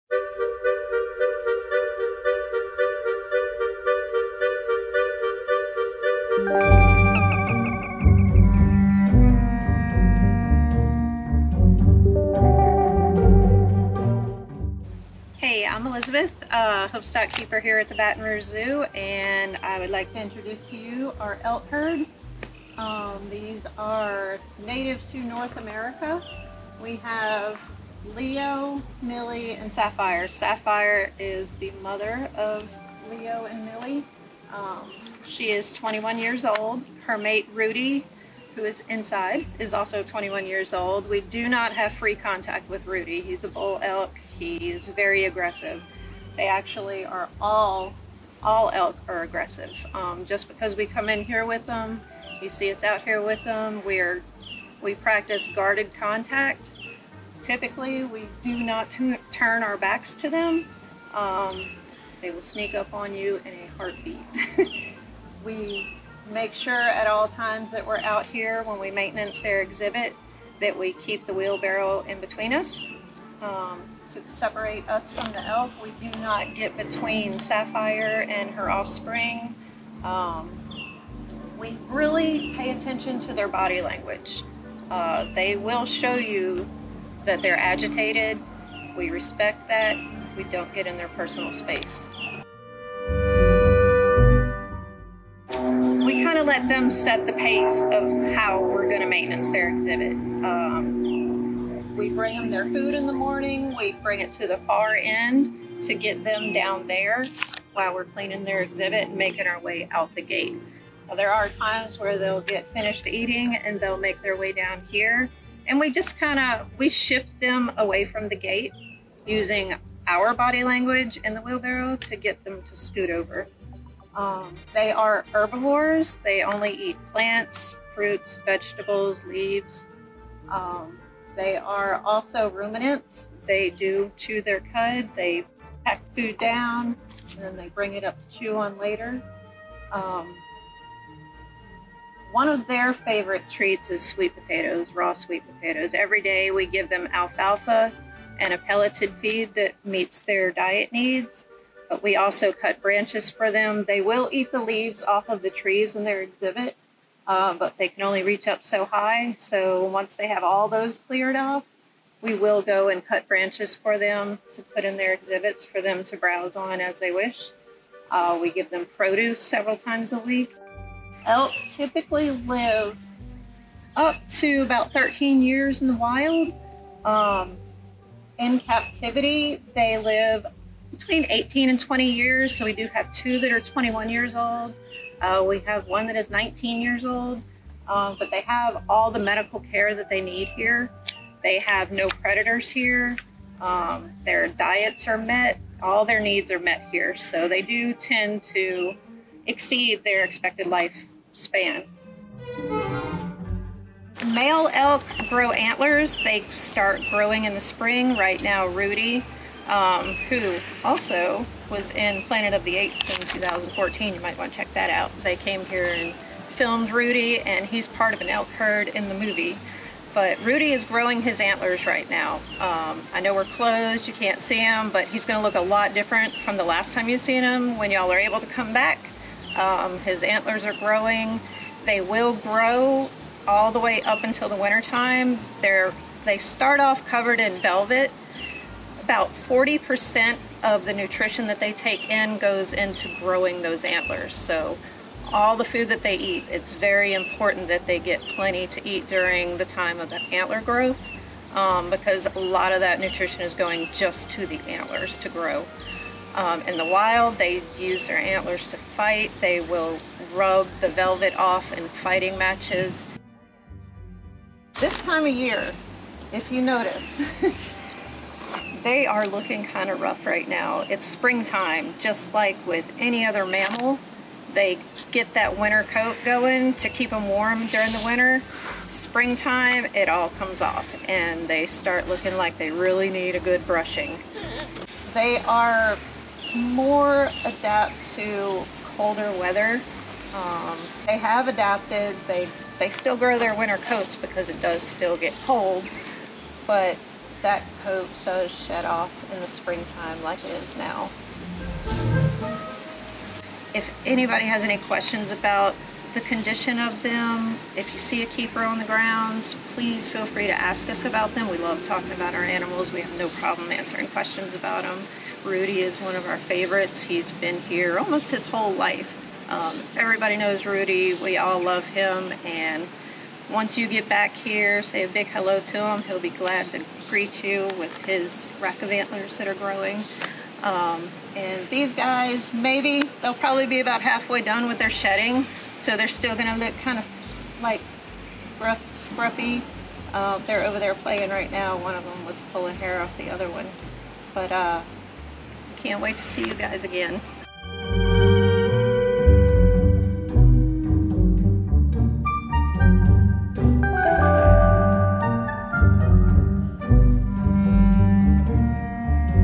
REFRESH: Animal Encounters at the Baton Rouge Zoo - Elk
One interesting fact about bull elks – when they grow antlers, it takes a ton of energy to produce the rack. Zookeeper Elizabeth introduces us to this family of elk – including one who had his own 15 minutes of fame!